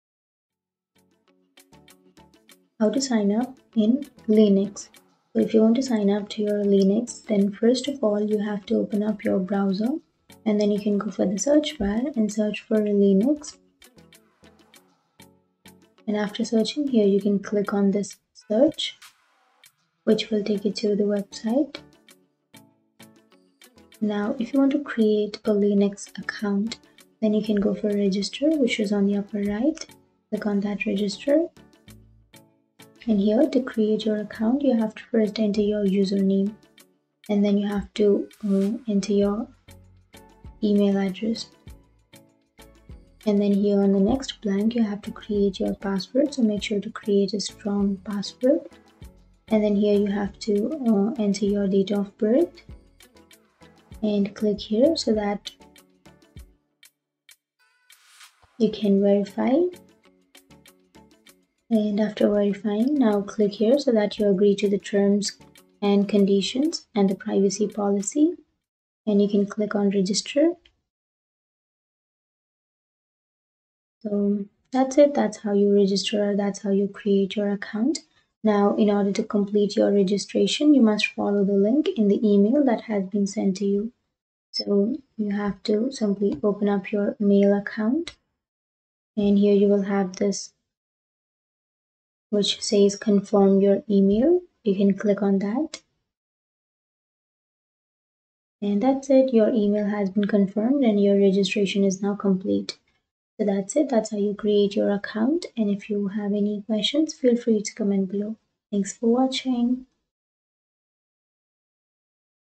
How To Signup Linux
How To Signup In Linux

Delve into our comprehensive tutorial on how to sign up for Linux. Understanding the process of registering and setting up a new user account on Linux is essential for seamless system integration. Whether you're new to Linux or seeking to create a new user account, our step-by-step guide covers the intricacies of Linux account creation, ensuring a smooth setup process. Join us to explore the nuances of Linux signup, making your system entry effortless and efficient.